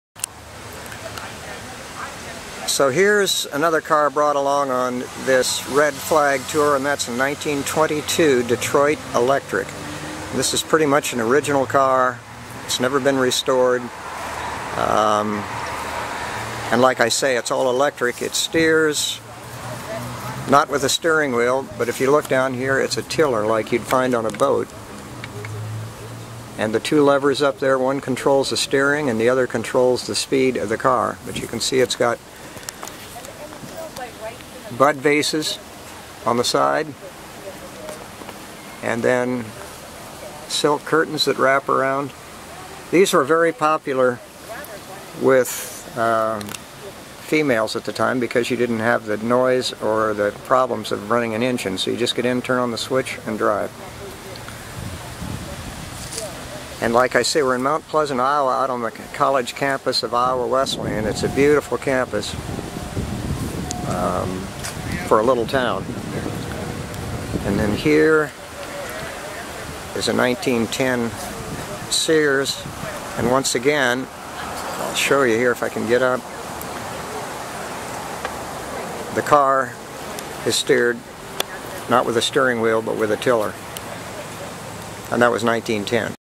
1922 Detroit Electric
!922 Detroit Electric on the 2013 Iowa Red Flag Horseless Carriage Tour. The car completed 3 days of touring at about 100 miles per day. not bad for a 1922 electric car.

The car is completely original, down to the bud vases in the interior.